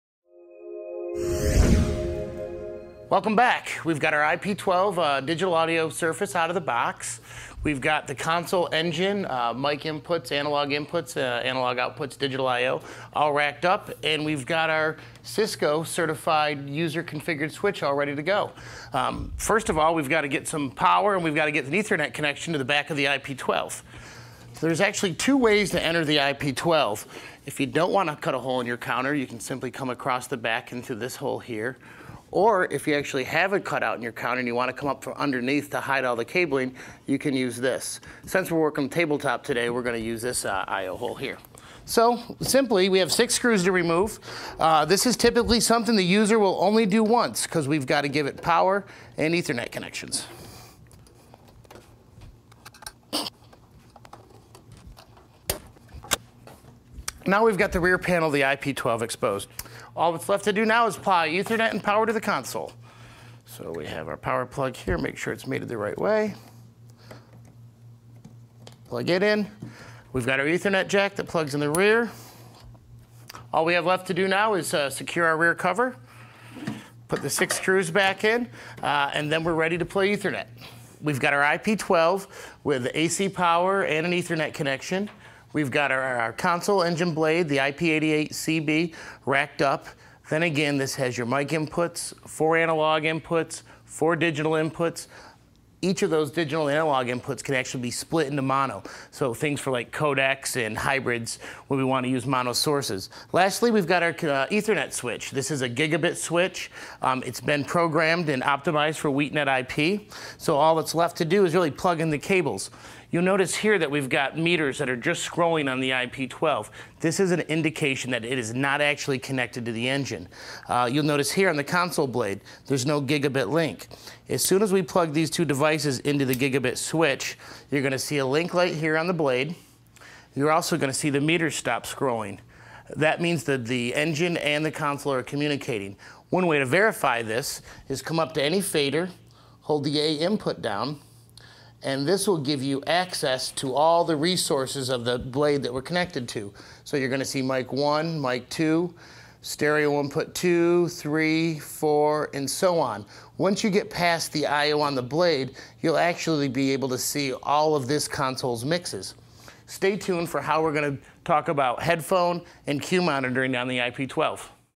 Setting Up the IP-12, Part 2: Cabling Up and Powering Up
How easy is it to set up a typical Wheatstone AoIP console? Wonder no more. In this series of videos, Jay Tyler takes you through the whole process in surprisingly little time! Here's part two.